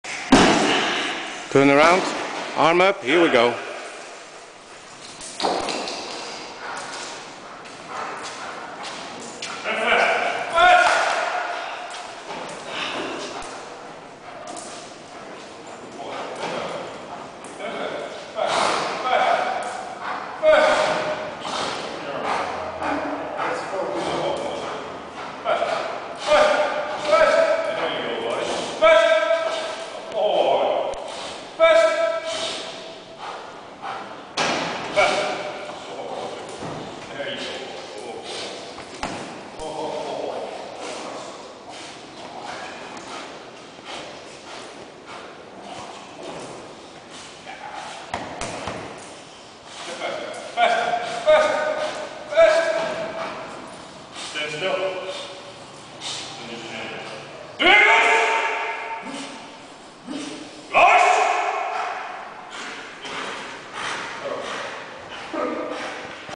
Personal Protection Dog Practicing Bite Work Video
Scott's K9 trainer practicing bite work in warehouse during initial training.